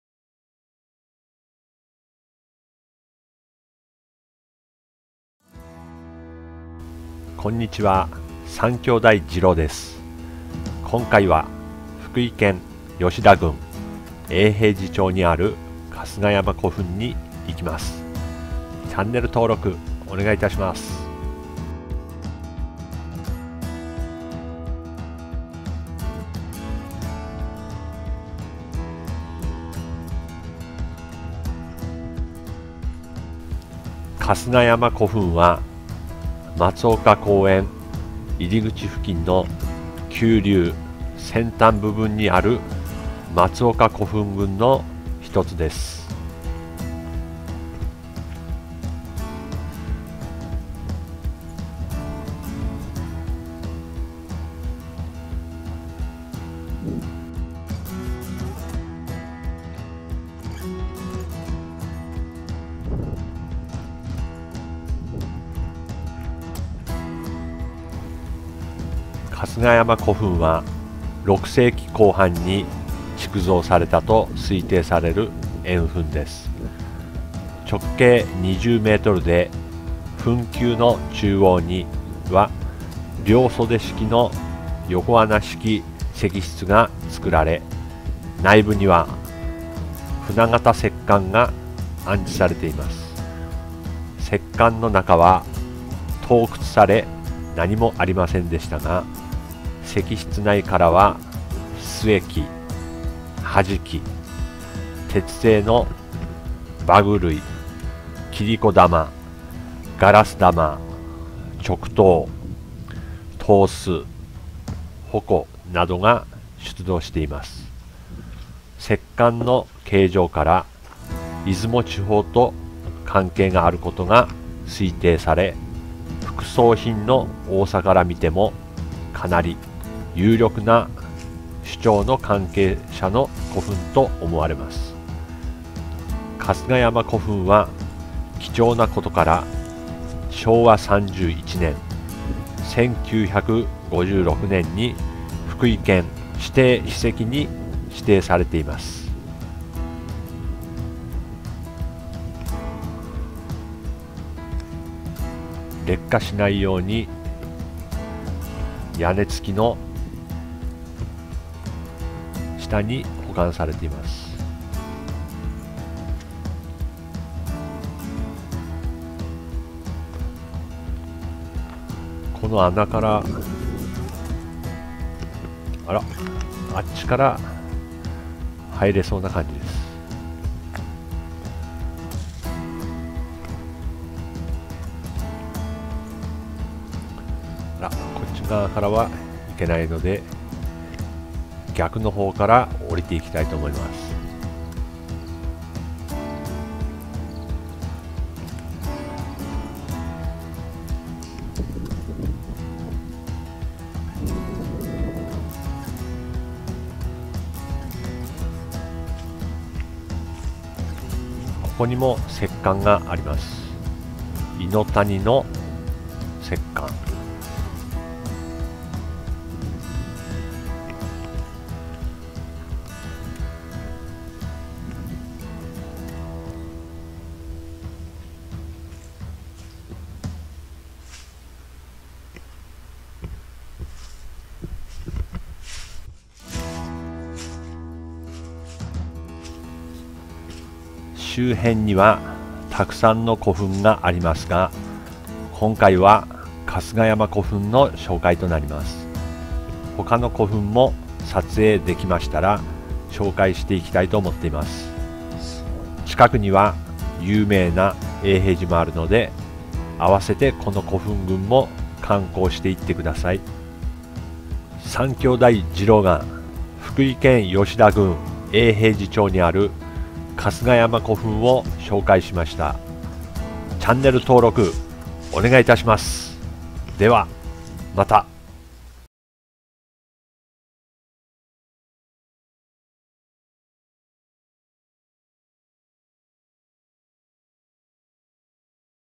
福井県PR動画 ローカルスポット 春日山古墳 #21
ブログ作りました( ^ω^ )

福井県のあまり知られていない観光地を紹介する動画です。

春日山古墳は松岡公園入口付近の丘陵先端部分にある松岡古墳群の一つです。

春日山古墳は６世紀後半に築造されたと推定される円墳です。長径２０ｍで、墳丘の中央には両袖式の横穴式石室が造られ、内部には舟形石棺が安置されています。石棺の中は盗掘され何もありませんでしたが、石室内からは須恵器、土師器、鉄製の馬具類、切子玉、ガラス玉、直刀、刀子、鉾などが出土しています。石棺の形状から出雲地方と関係があることが推定され、副葬品の多さから見ても、かなり有力な首長の関係者の古墳と思われます。春日山古墳は貴重な事から昭和３１年（１９５６）に福井県指定史跡に指定されています。

周辺にはたくさんの古墳がありますが今回は春日山古墳の紹介となります。
他の古墳も撮影出来ましたら紹介していきたいと思っています。

近くには有名な永平寺もあるので、合わせてこの古墳群も観光して行ってください。
今回は福井県吉田郡永平寺町にある松岡公園入口にある春日山古墳に行ってきました。近くには有名な永平寺がありたくさんの観光地があります。皆さんも是非遊びに来てください。